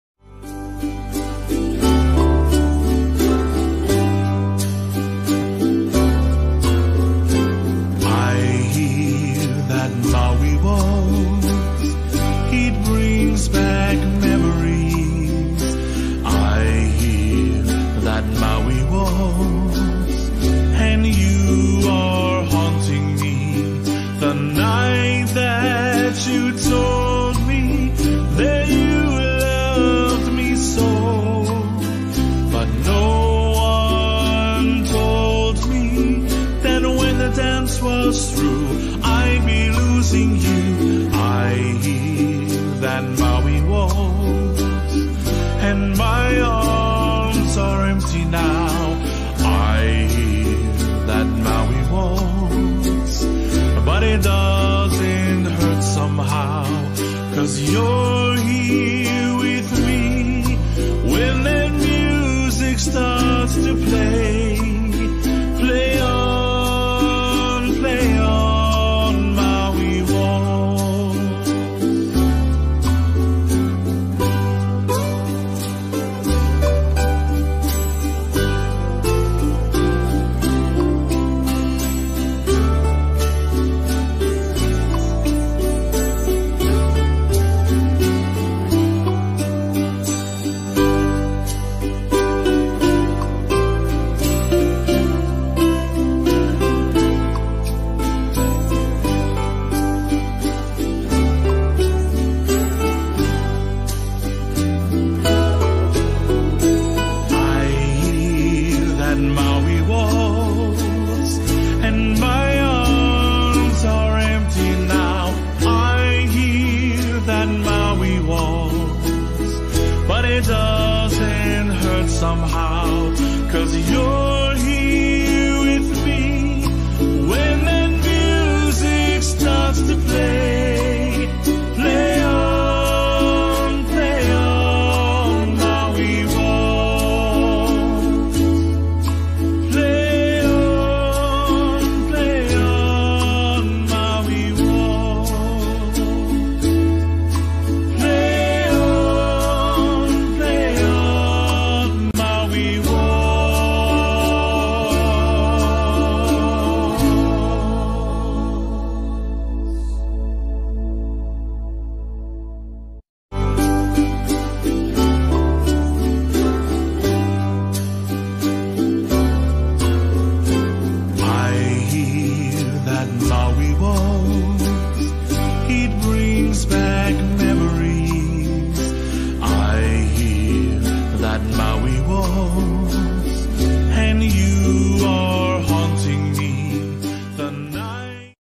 THE MAUI WALTZ- line dance ( Sally Hung)
Choreographer by Sally Hung Taiwan  Septemver 2022

24 C   4 Wall  Beginner ,No tag,No Restart